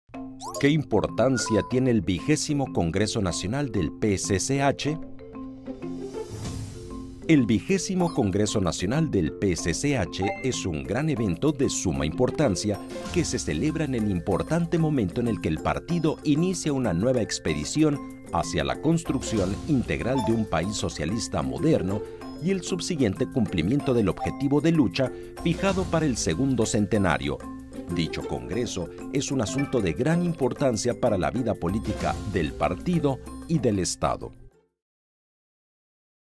¿Qué importancia tiene el XX Congreso Nacional del PCCh?
El XX Congreso Nacional del PCCh es un gran evento de suma importancia que se celebra en el importante momento en el que el Partido inicia una nueva expedición hacia la construcción integral de un país socialista moderno y el subsiguiente cumplimiento del objetivo de lucha fijado para el segundo centenario, dicho congreso es un asunto de gran importancia para la vida política del Partido y del Estado.